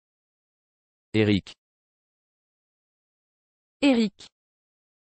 How to spell Éric
This video will teach you how to spell Éric, the French version of the popular scandinavian name Erik.